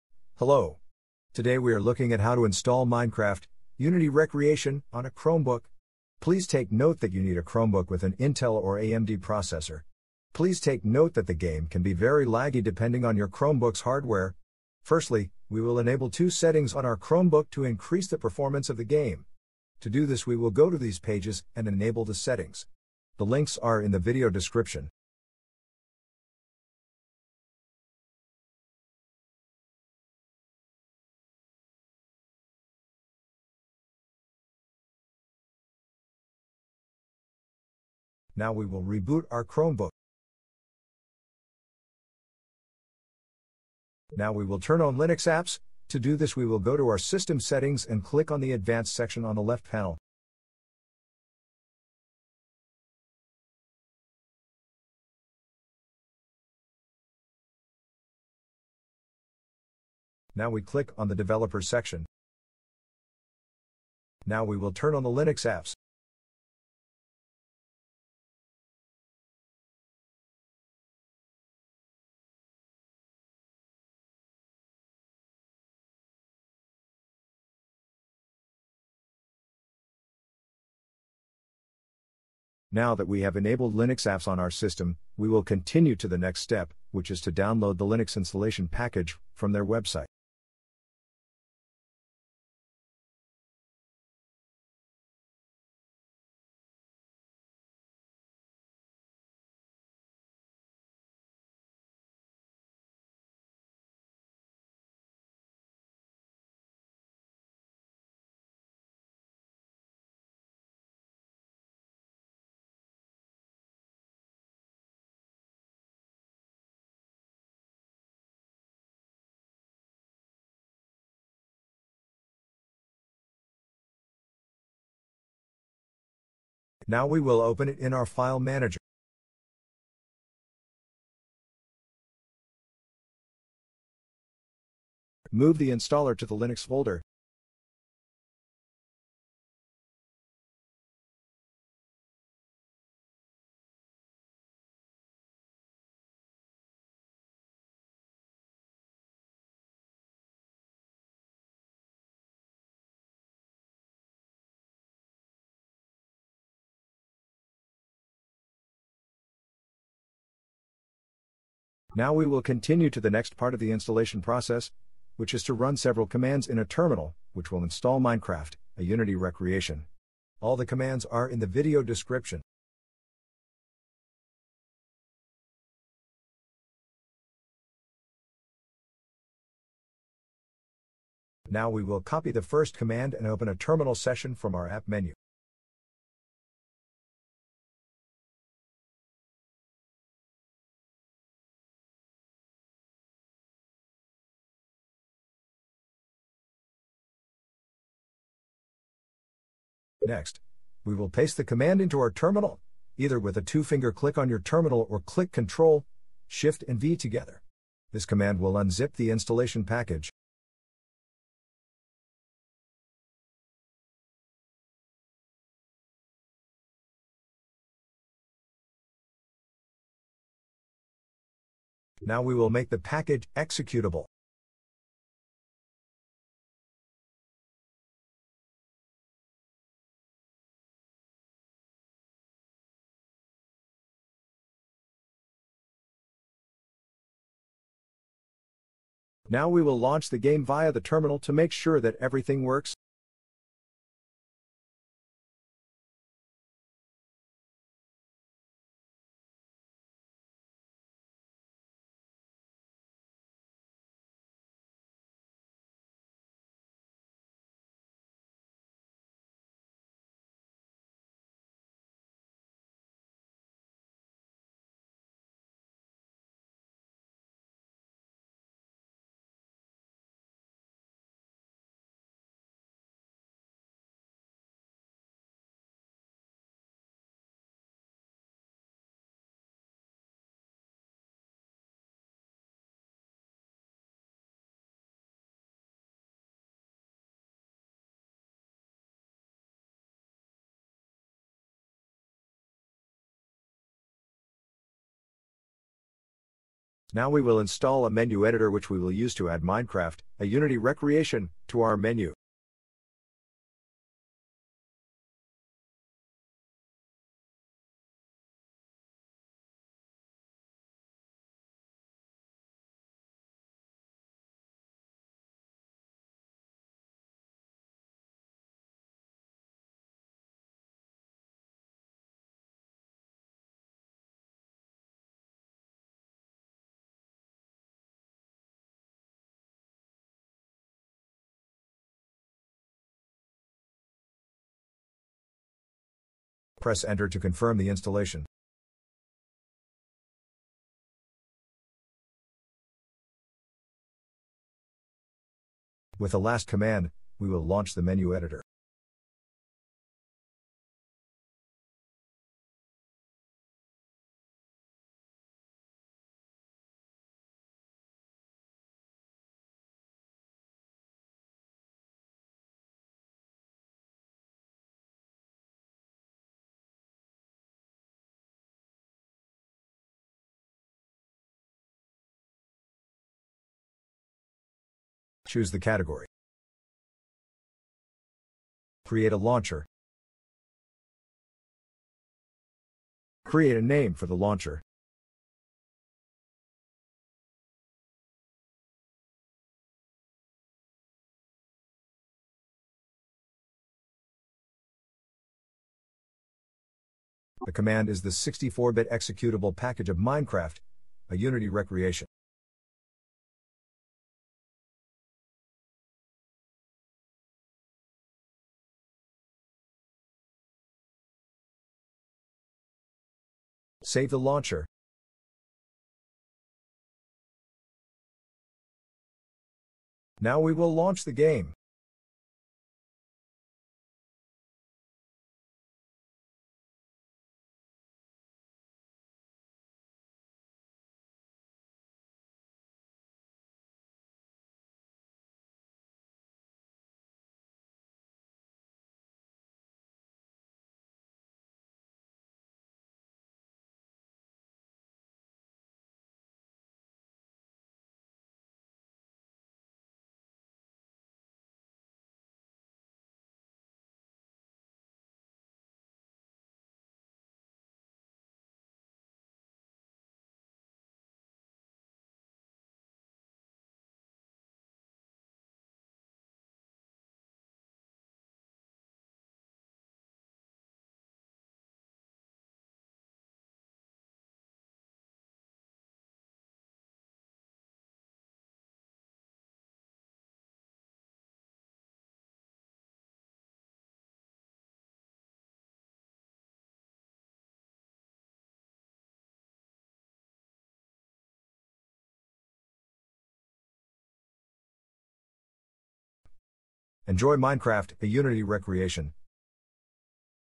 How to install Minecraft (Unity Recreation) on a Chromebook
In this video, we are looking at how to install MCreator on a Chromebook.